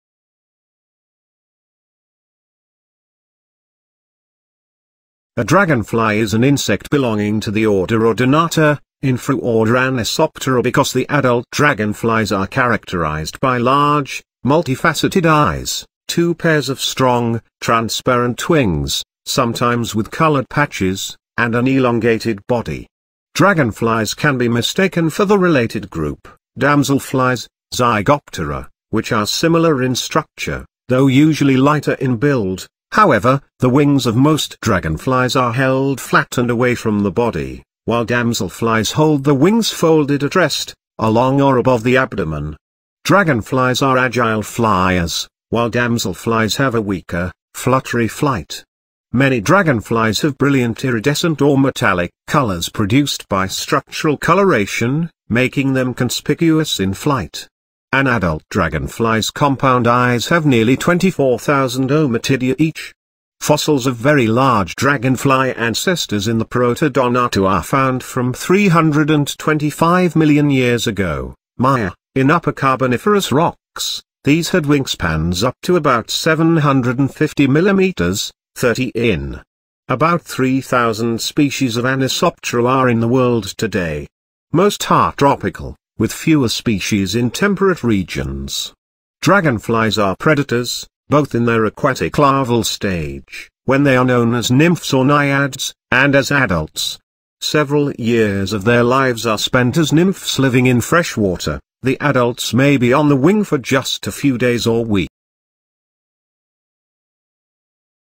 Interesting facts about dragonfly by weird square
A dragonfly is an insect belonging to the order Odonata, infraorder Anisoptera because the Adult dragonflies are characterized by large, multifaceted eyes, two pairs of strong, transparent wings, sometimes with coloured patches, and an elongated body. Dragonflies can be mistaken for the related group, damselflies (Zygoptera), which are similar in structure, though usually lighter in build; however, the wings of most dragonflies are held flat and away from the body, while damselflies hold the wings folded at rest, along or above the abdomen. Dragonflies are agile fliers, while damselflies have a weaker, fluttery flight. Many dragonflies have brilliant iridescent or metallic colours produced by structural coloration, making them conspicuous in flight. An adult dragonfly's compound eyes have nearly 24,000 ommatidia each.
Fossils of very large dragonfly ancestors in the Protodonata are found from 325 million years ago (Mya) in Upper Carboniferous rocks; these had wingspans up to about 750 mm (30 in). About 3000 species of Anisoptera are in the world today. Most are tropical, with fewer species in temperate regions.
Dragonflies are predators, both in their aquatic larval stage, when they are known as nymphs or naiads, and as adults. Several years of their lives are spent as nymphs living in fresh water; the adults may be on the wing for just a few days or weeks. They are fast, agile fliers, sometimes migrating across oceans, and are often found near water. They have a uniquely complex mode of reproduction involving indirect insemination, delayed fertilization, and sperm competition. During mating, the male grasps the female at the back of the head or on the prothorax, and the female curls her abdomen under her body to pick up sperm from the male's secondary genitalia at the front of his abdomen, forming the "heart" or "wheel" posture. Animal planet | Discovery | Netflix | Documentary | Animal World